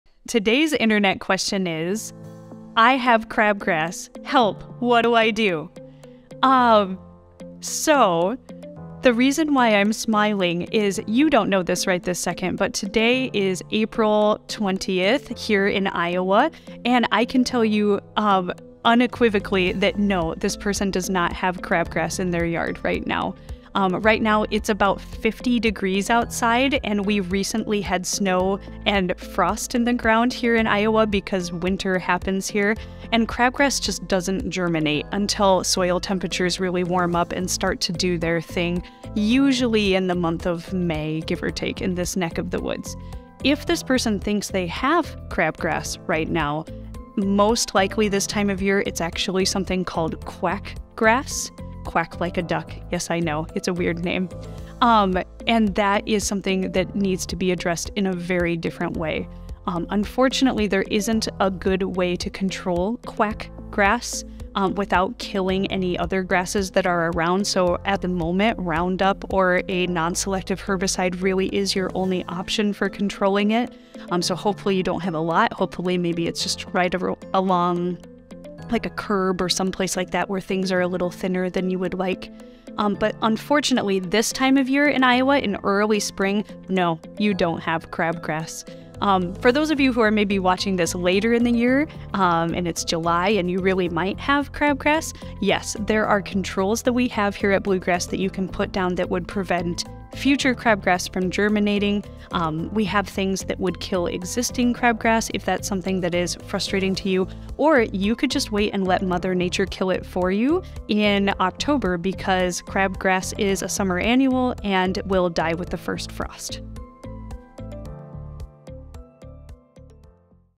Crabgrass vs Quackgrass During Early Spring in Iowa
We get a lot of questions about "crabgrass" in March, April, and early May in Iowa. It always surprises people to find out that the grassy weed they are noticing in their lawn in early spring is definitely not crabgrass. Learn more about quackgrass here!